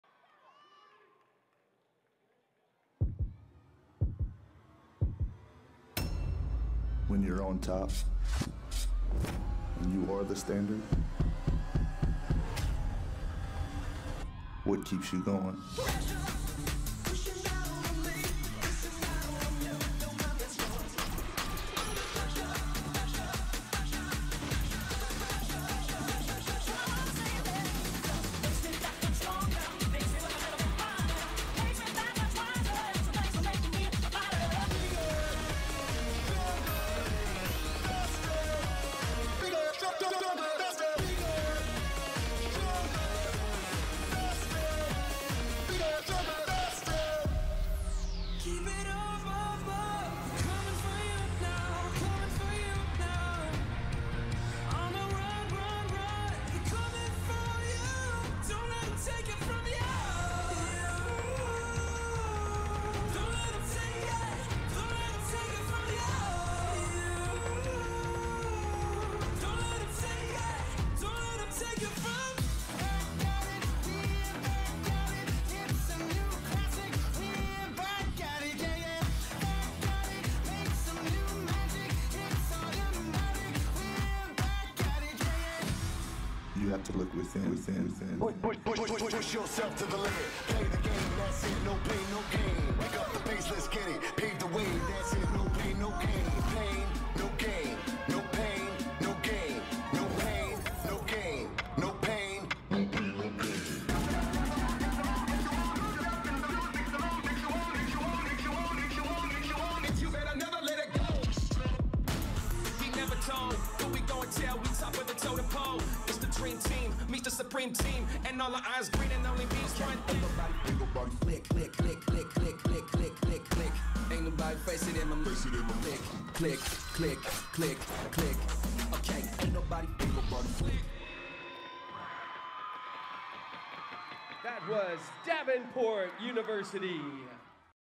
2024 DII TEAM PERFORMANCE NATIONAL CHAMPIONS
Choreography: BTUCK Choreography
Costume: Ignite Dance Connections
Coaching Staff: Courtney Houldsworth, Madison Letwinski, Morgan Bernstein, Madelyn McCallum, Kendall Chapple
Location: 2024 NDA/ NCA College Nationals | Daytona, FL